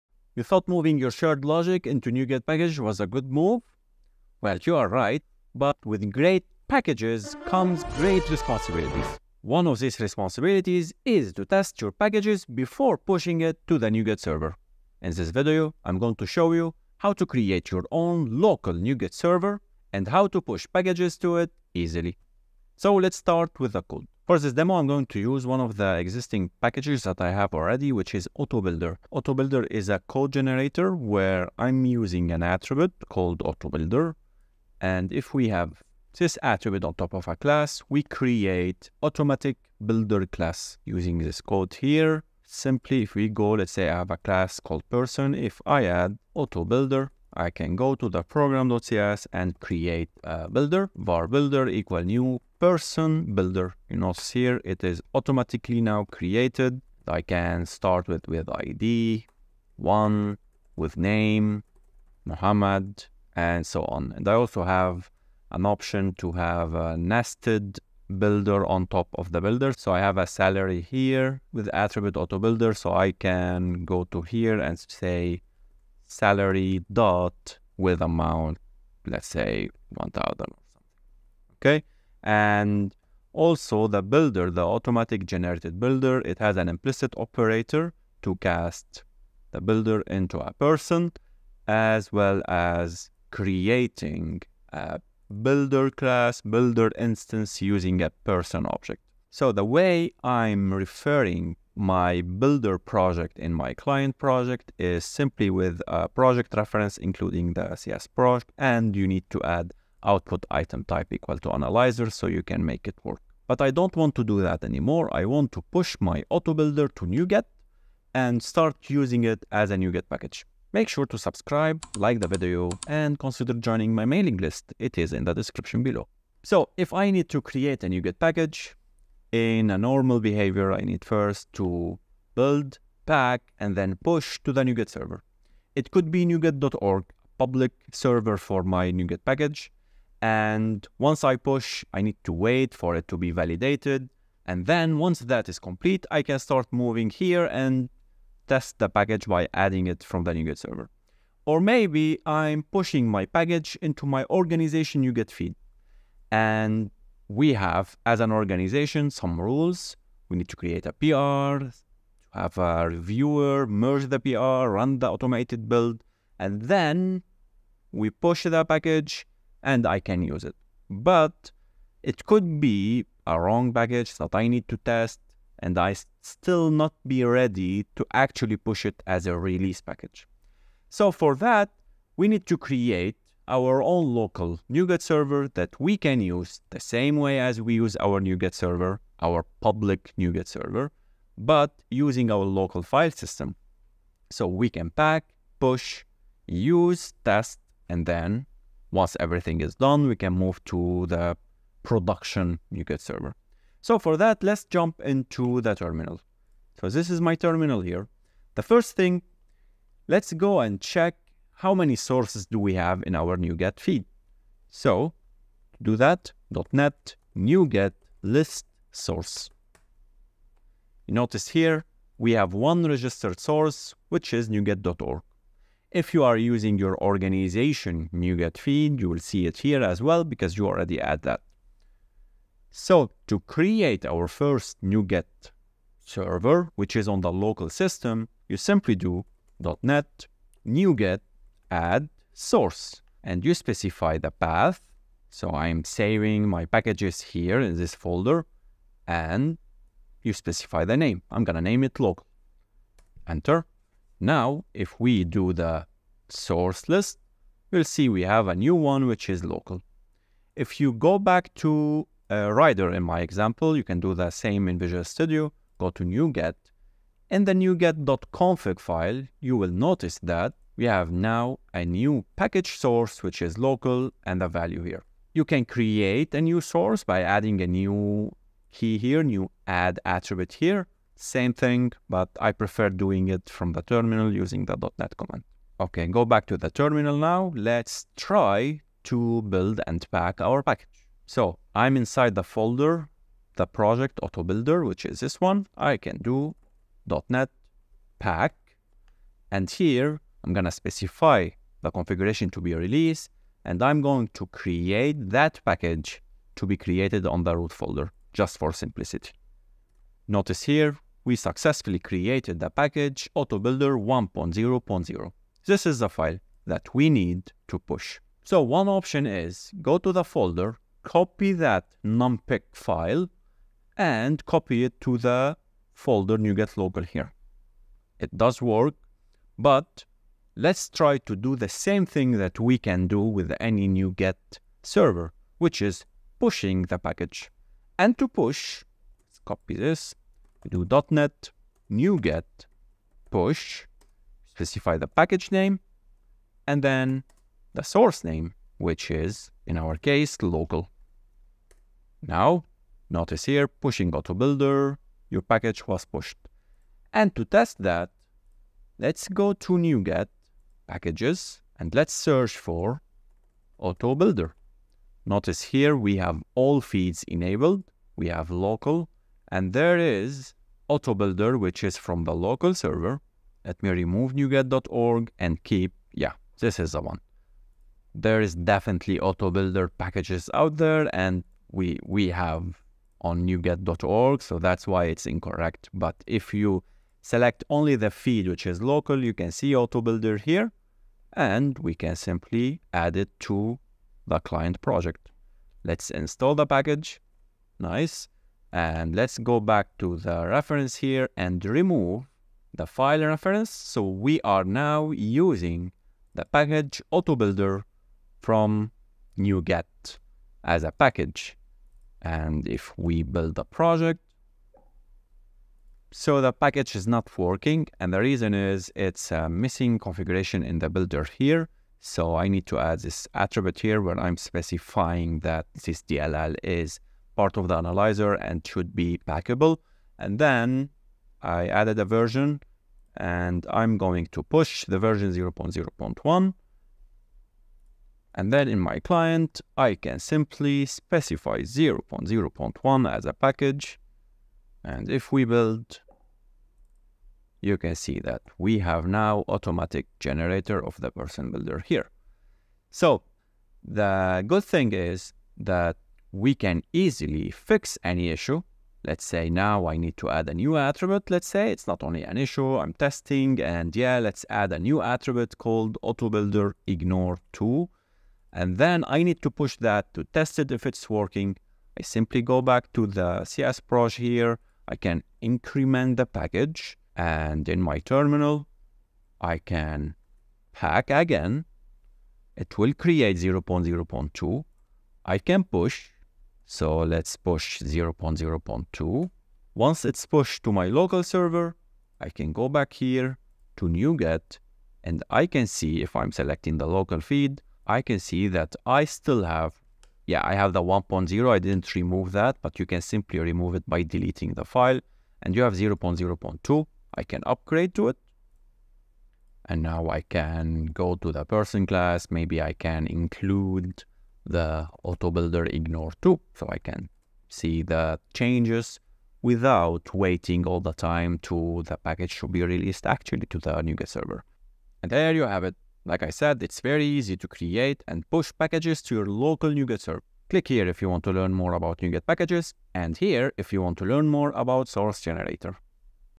How to Host Your NuGet Packages Locally | The Easy Way
Waiting for a NuGet package to be released and deployed can be frustrating. It’s helpful to have a way to test the package locally before pushing it online.

In this video, I’ll show you how to set up a Local NuGet Source

Chapters:
00:00 Intro
00:24 AutoBuilder
02:06 How To NuGet
05:39 Push And Test
08:45 Outro

Happy Coding!